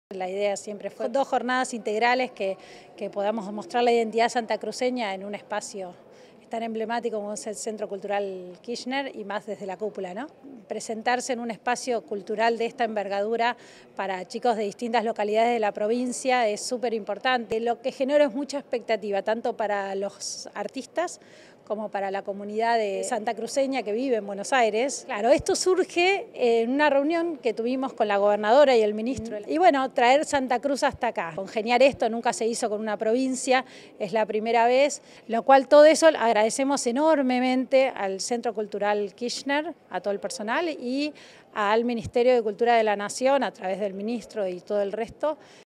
Campos: “La idea del evento fue mostrar la identidad santacruceña”
Así lo manifestó, la secretaria de Estado de Responsabilidad Social, Roció Campos, este jueves en el marco de la segunda jornada de la Temporada Turística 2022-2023 "Anímate Santa Cruz - Cultura y Turismo” llevada a cabo en el Centro Cultural Kirchner (CCK) de la Ciudad Autónoma de Buenos Aires.

En este sentido, Rocío Campos destaco que “la idea de estas dos jornadas es que podamos mostrar la identidad santacruceña en un espacio tan emblemático como es el Centro Cultural Kirchner y más desde la cúpula. Por eso estamos muy contentos en este sentido.”.

“Presentar en un espacio cultural de esta envergadura para los artistas de distintas localidades de la provincia es importante porque este tipo de eventos ha generado muchas expectativas tanto para ellos como a la comunidad santacruceña que vive aquí en Buenos Aires”, señaló.

Por último, la secretaria de Estado de Responsabilidad Social explicó cómo surgió la idea del evento. “Esto surgió mediante una reunión que tuvimos con la gobernadora Alicia Kirchner y el ministro de Cultura de la Nación, Tristán Bauer para traer a la provincia a este lugar. Nunca se hizo un evento de este tipo con ninguna provincia, es la primera vez que se realiza, es por ello que, agradecemos al ministro de Cultura y a todo el personal del CCK”.